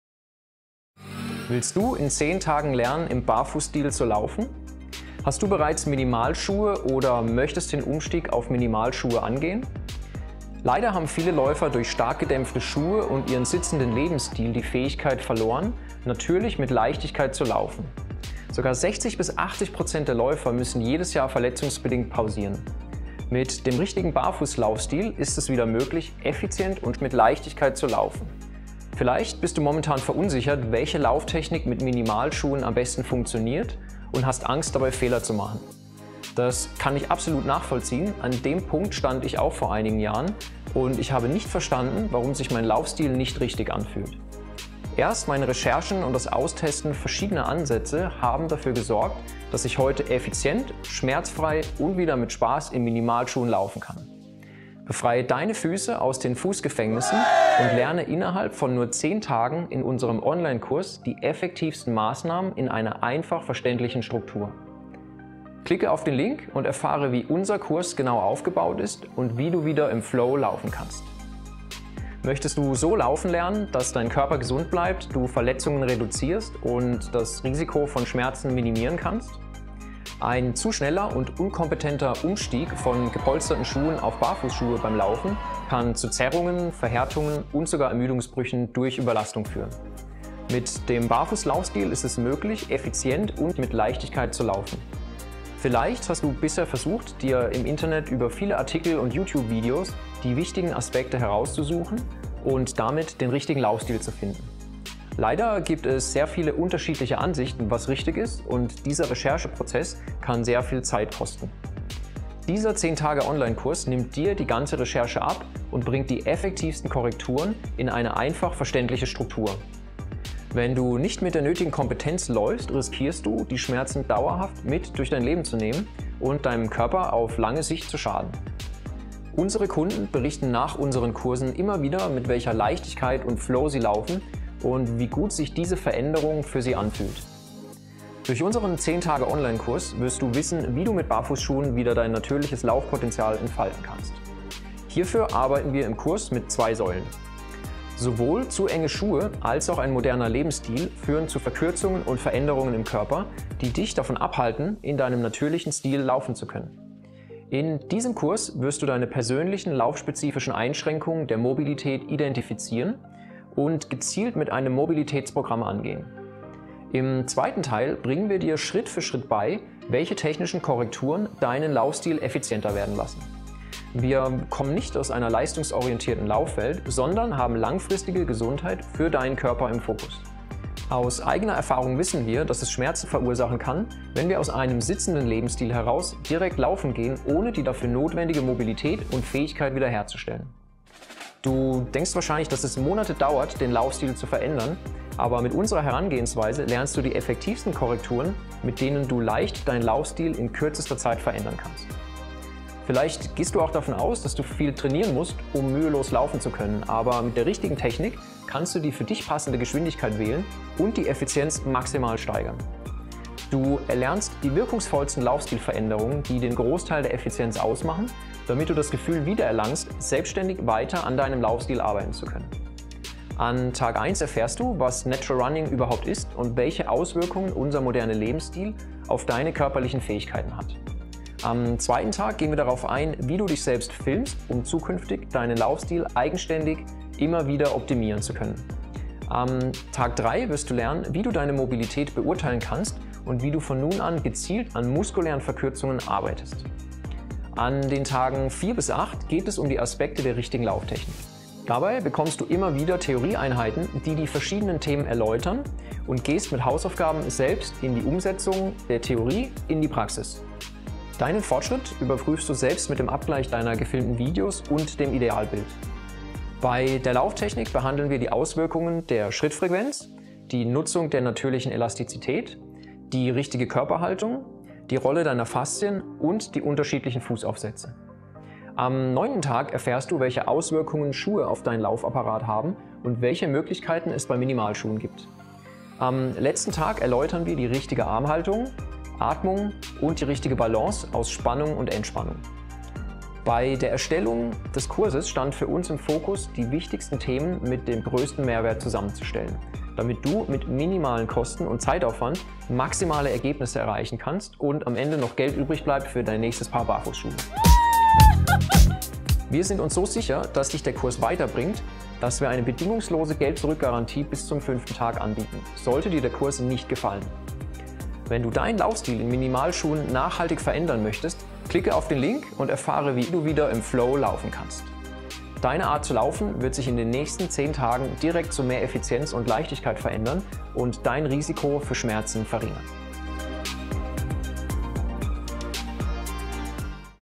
Natural Running Online Kurs - Lerne Lauftechnik und Mobilität
Lerne in 10 Tagen mit Barfußschuhen effizient zu laufen!

Stellst du dir die Frage wie du mit Minimalschuhen richtig läufst?

Willst du deine Füße aus den Fußgefängnissen befreien und schmerzfrei und unbeschwert laufen?

In diesem Kurs lernst du die wichtigsten Aspekte des Natürlichen Laufens und erfährst, wie du die Veränderungen des sitzenden Lebensstils wieder rückgängig machen kannst.